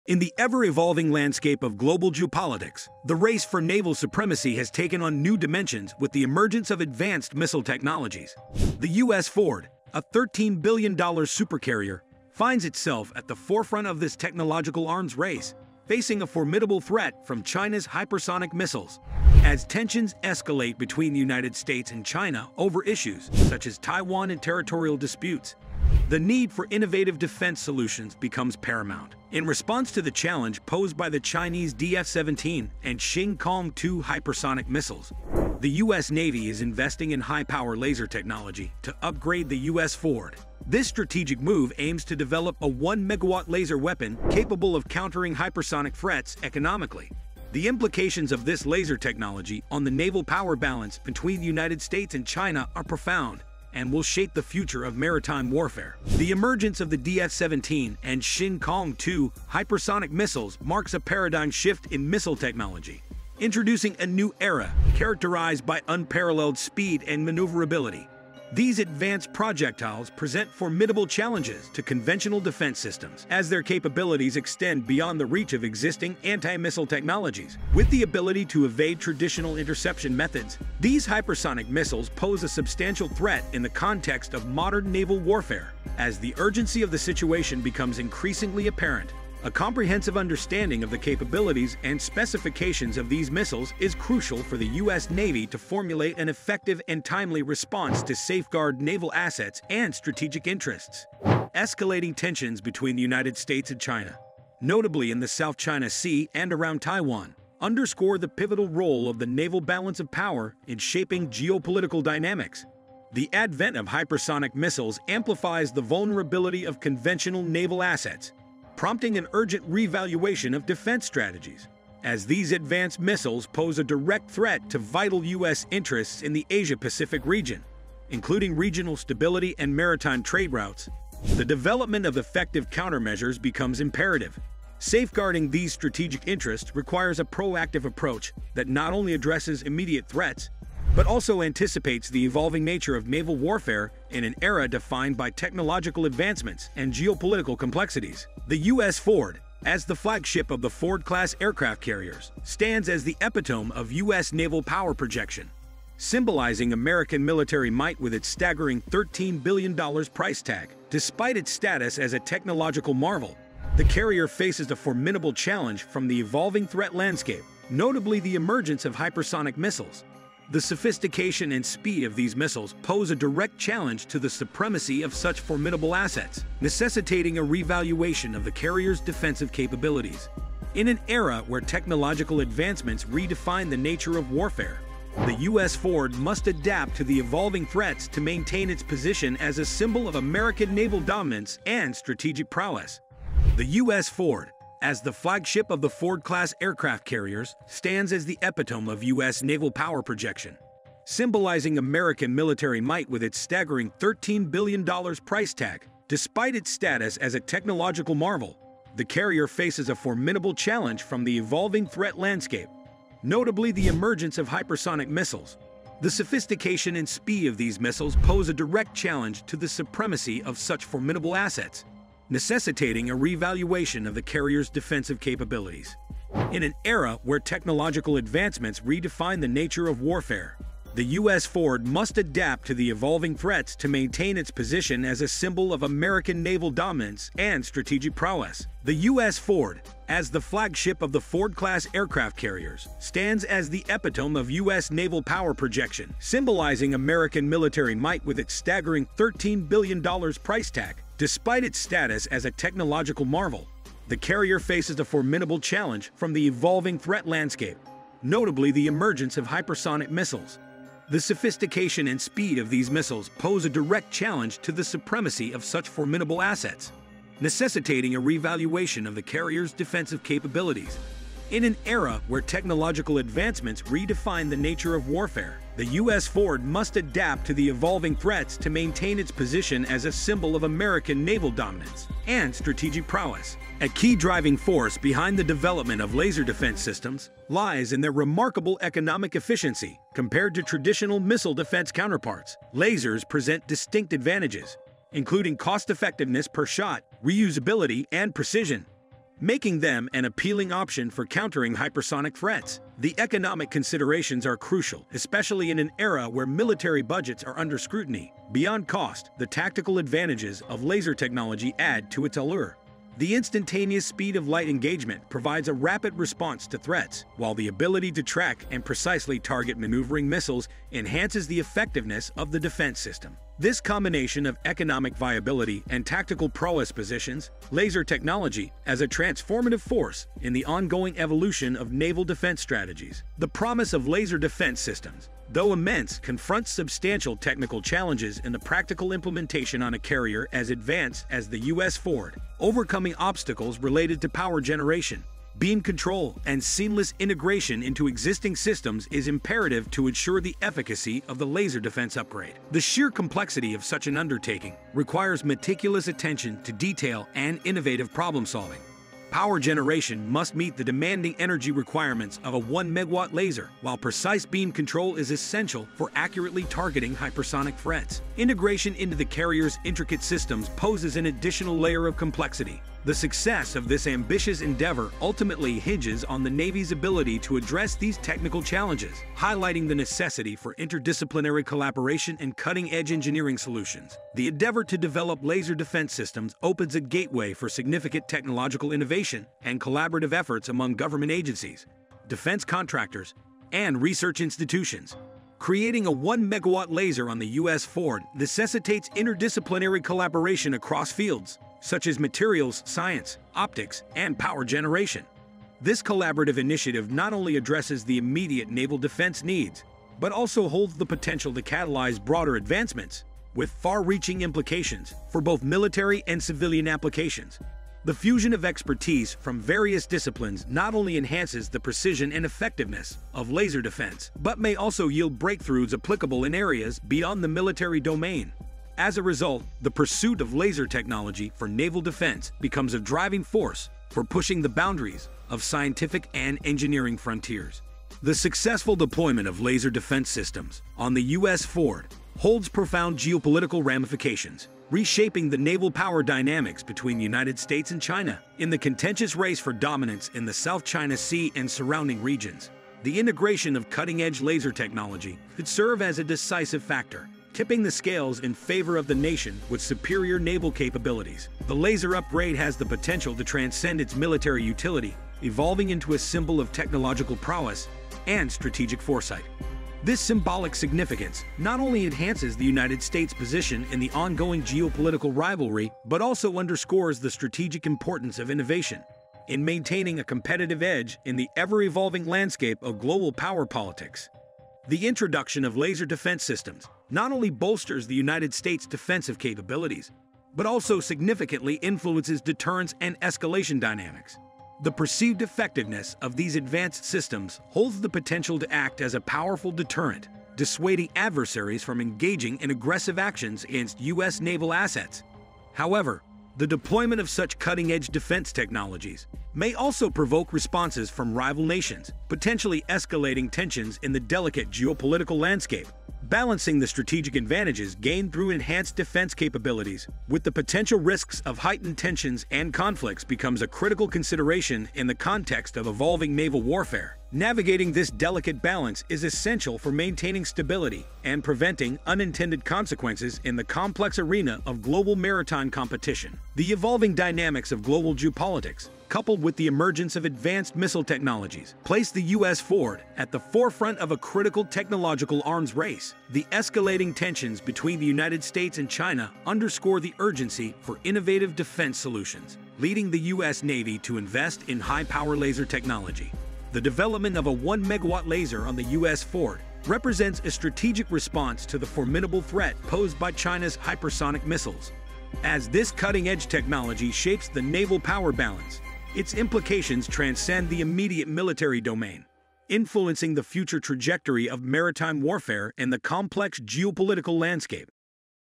Untold Secrets of US Navy's Laser Weapon: World Is Not Prepared!
In the ever-evolving landscape of global geopolitics, the race for naval supremacy has taken on new dimensions with the emergence of advanced missile technologies. The USS Ford, a $13 billion supercarrier, finds itself at the forefront of this technological arms race, facing a formidable threat from China's Hypersonic missiles. As tensions escalate between the United States and China over issues such as Taiwan and territorial disputes, the need for innovative defense solutions becomes paramount. In response to the challenge posed by the Chinese DF-17 and Shing Kong 2 Hypersonic missiles, the U.S. Navy is investing in high-power laser technology to upgrade the USS Ford. This strategic move aims to develop a one-megawatt laser weapon capable of countering Hypersonic threats economically. The implications of this laser technology on the naval power balance between the United States and China are profound and will shape the future of maritime warfare.